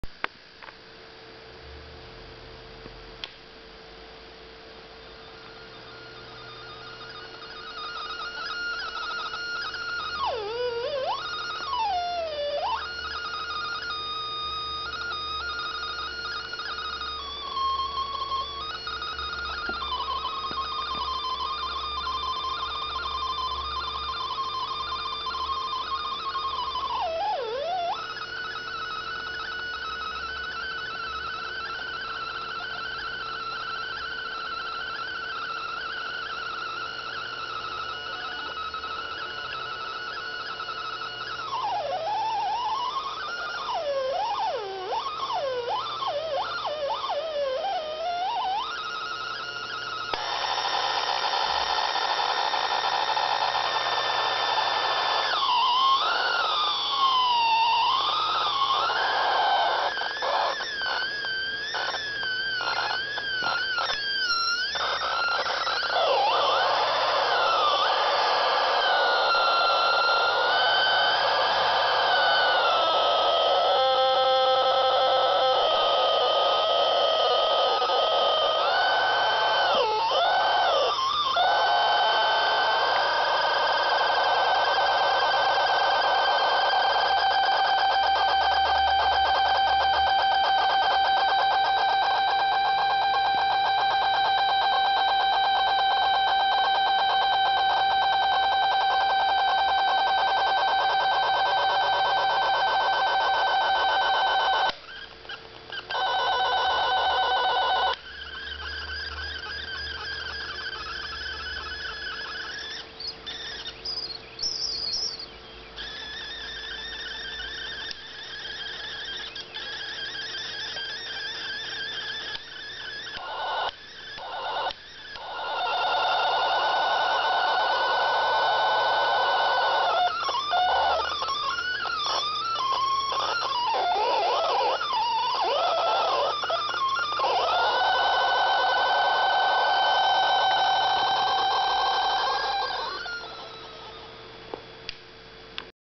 ThingamaKit sound robot in action
I assembled this little guy for a friend of mine. This video shows a first impression how it sounds.